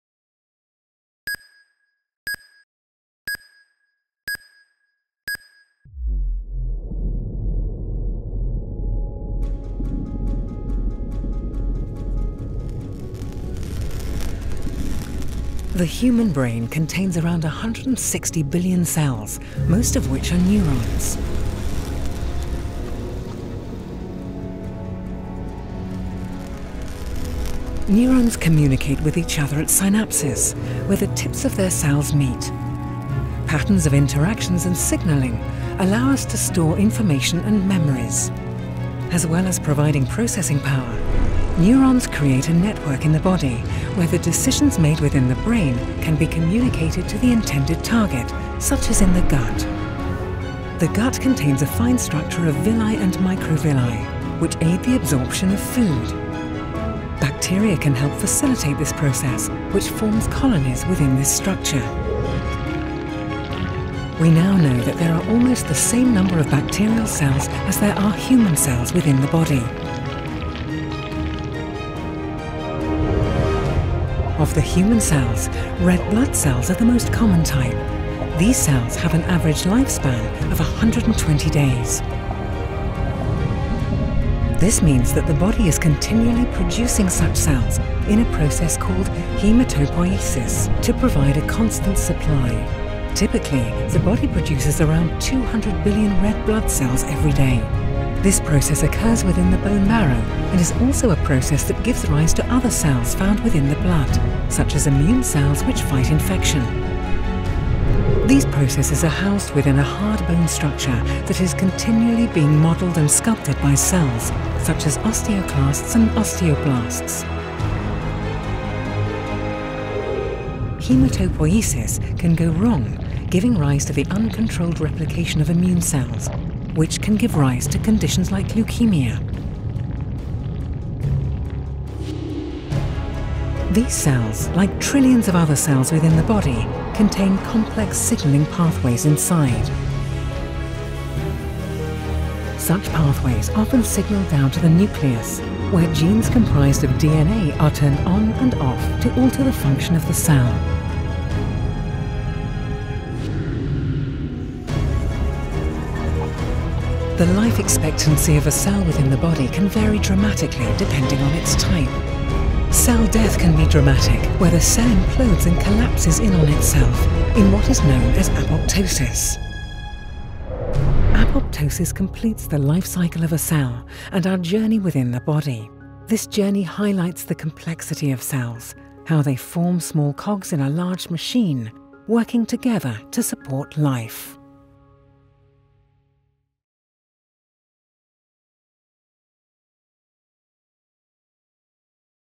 Intercellular – An Interbody VR Experience
Intercellular – learn more about your body.

Intercellular takes you on a journey inside the human body. Discover a variety of cells performing different functions, from neurons in the brain to bacteria within the gut and everywhere in-between. Travel along inside a cell and explore the nucleus to reveal DNA, the code that makes us who we are.

For the best experience please download the Random42 app and use a VR viewer with your phone.